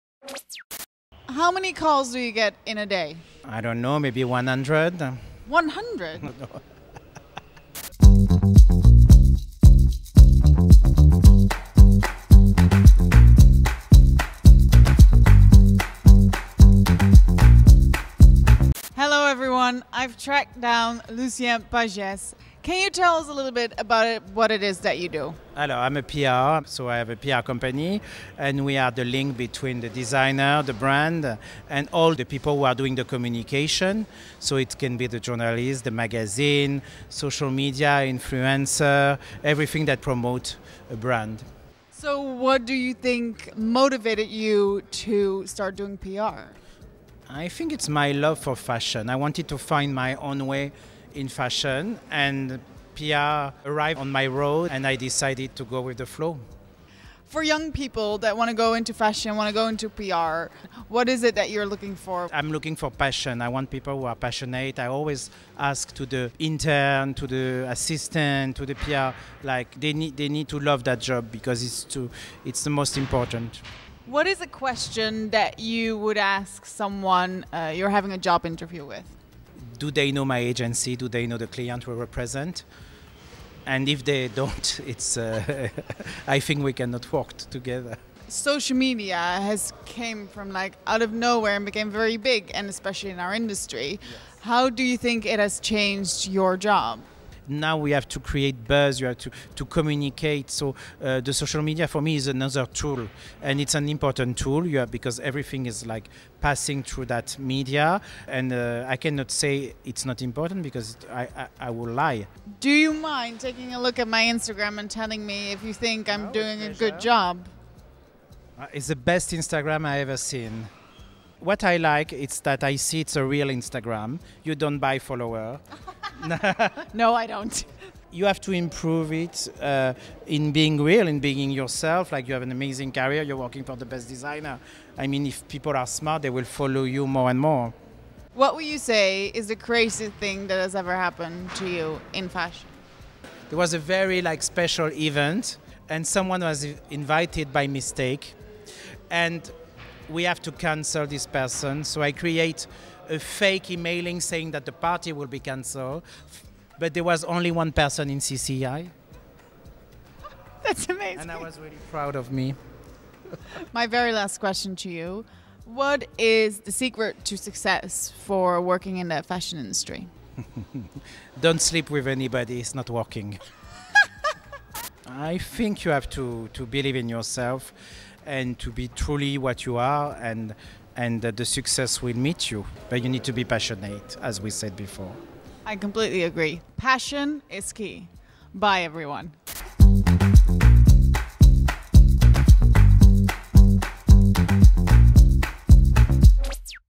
Rianne Van Rompaey asks Lucien Pagès how to get a job in fashion PR & Communications | Vogue Paris
From who gets to interview a designer to who sits where at a show, in the exclusive world of fashion, it's the PRs who hold the keys. Lucien Pagès is one of the top fashion communication directors in Paris, working with the biggest brands in the world. Rianne van Rompaey caught up with him at Paris Fashion Week to find out what it takes to succeed in such a fast-paced world.

Starring Lucien Pagès & Rianne van Rompaey
Directors - Jules Jarossay & Valentine Vernier
Producer - La Maison Noire
Journalist - Josh Arnold
Editor-in-chief - Jennifer Neyt